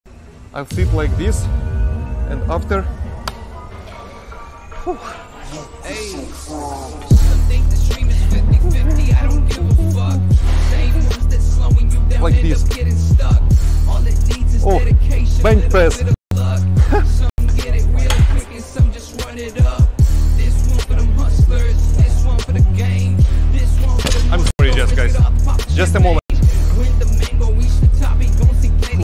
10 Tips for an Effective Benjpress Workout
10 Tips for an Effective Benjpress Workout
Unlock the secrets to a powerful Benjpress workout with our exclusive guide featuring '10 Tips for an Effective Benjpress Workout'! Whether you're a seasoned lifter or just starting out, these expert tips will elevate your bench press game to new heights. From perfecting your form to optimizing your routine, each tip is carefully crafted to help you achieve maximum results and build strength like never before. Say goodbye to plateaus and hello to gains – click now to discover the keys to a stronger, fitter you!

✅-- If you need any help to Edit your Videos  - Check the Links Bellow ⤵️

✅ Recommendation videos!!